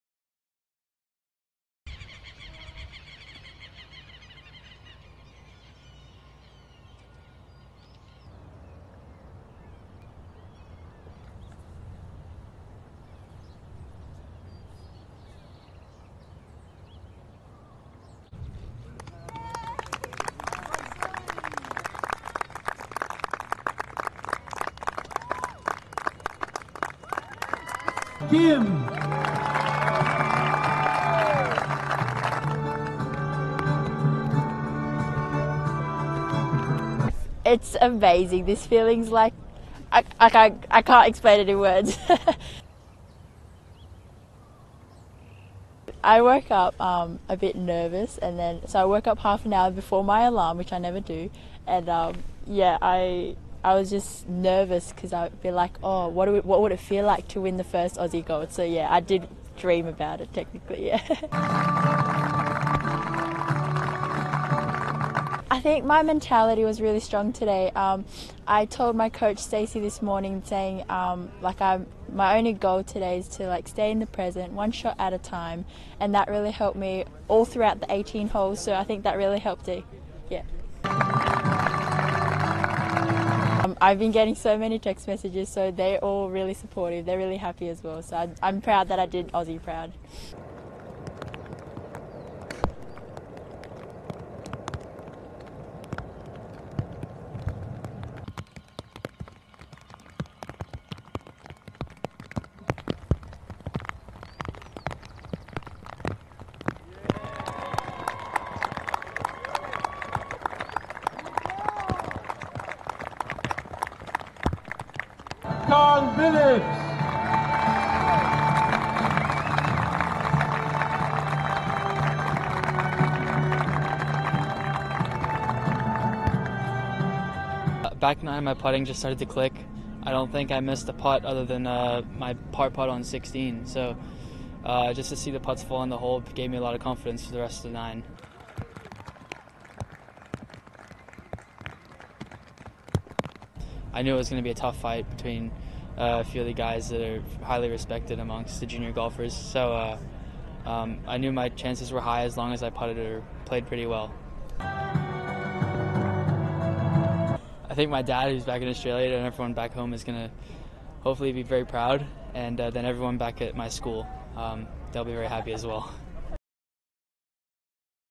AUSTRALIA DOMINATE IN GOLF WITH GOLDS IN MEN AND WOMEN’S STROKE PLAY AT BUENOS AIRES 2018
Australia won gold in both men’s and women’s golf events at the Youth Olympic Games Buenos Aires 2018.
The stroke play events took place at the Hurlingham Club In the Argentine capital, Australia’s Kim Grace, 17, took gold with a final round score of 71, two under par for her final round.
Shooting one over par overall after three rounds, Grace comfortably finished ahead of Yuka Saso (+4) from the Philippines and Austria’s Emma Spitz (+4).
In the men’s competition, Australia’s double-gold was confirmed when Karl Vilips, 17, shot a final round 69 to finish four under par to beat Akshay Bhatia from the United States of America and the Netherlands’ Jerry Ji who claimed silver and bronze respectively.
“Courtesy of The International Olympic Committee”